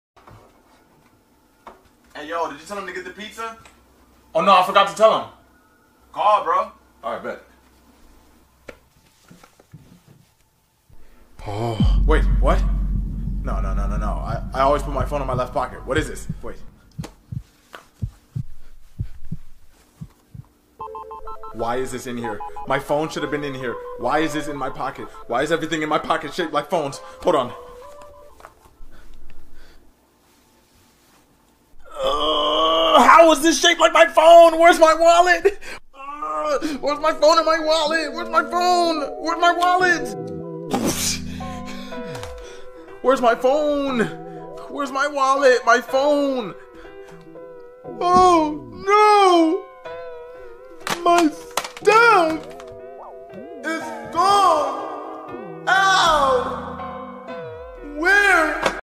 Oriole's phone is missing :( ¦ animatic
This may be a lot different from what I always make but it was fun to make:D

 my friends OCs are features in the video

Short hair girl- Maya
Straight long hair girl- Oriole
Guy- Lark

And the one in a ponytail is mine:D Plover

  Reaching your pocket and NOT finding
                your phone